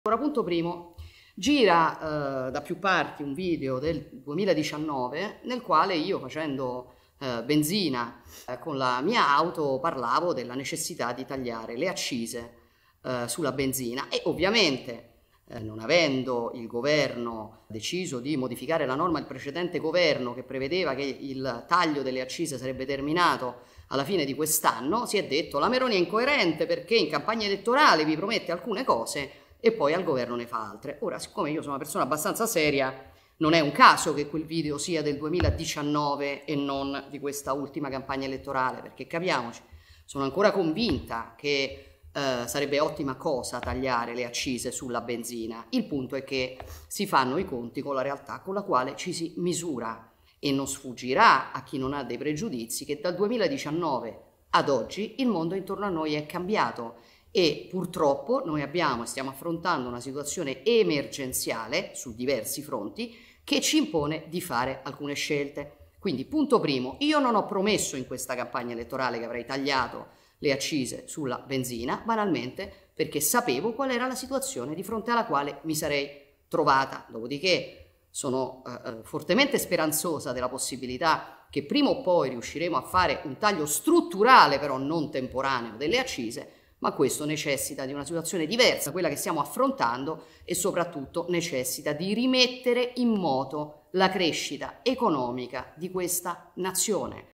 Carburanti, Meloni: «Giusto tagliare le accise, ma ora non è possibile»
«Penso ancora che sia giusto il taglio delle accise, ma si fanno i conti con la realtà e purtroppo stiamo affrontando una situazione emergenziale che ci impone di fare alcune scelte» lo sostiene il presidente del Consiglio, Giorgia Meloni, in una diretta sui social network riguardo il caro carburanti.